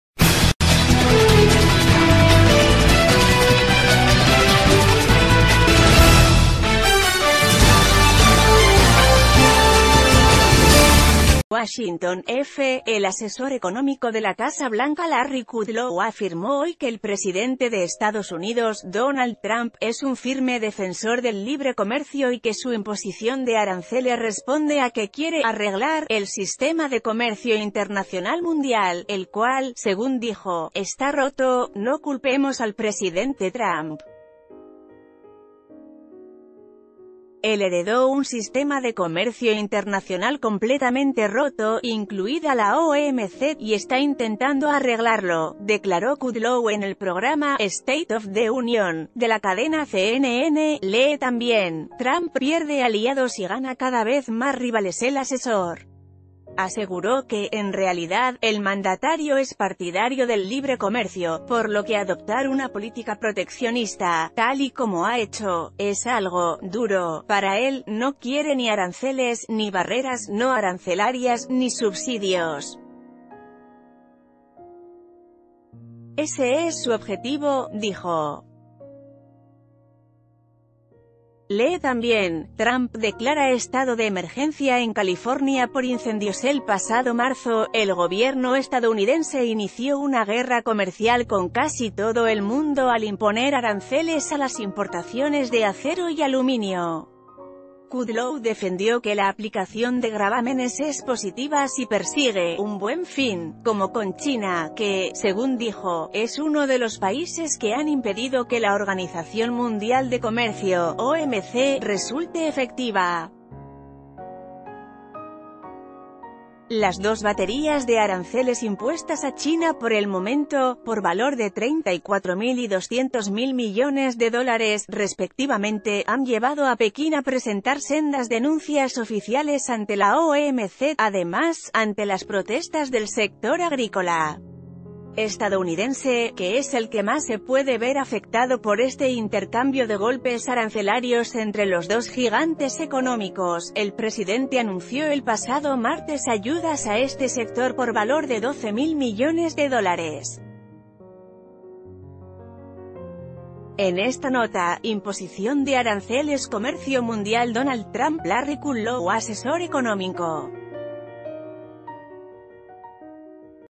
Trump quiere "arreglar" el comercio mundial: Kudlow
Washington (EFE).- El asesor económico de la Casa Blanca Larry Kudlow afirmó hoy que el presidente de Estados Unidos, Donald Trump, es un firme defensor del libre comercio y que su imposición de aranceles responde a que quiere "arreglar" el sistema de comercio internacional mundial, el cual, según d...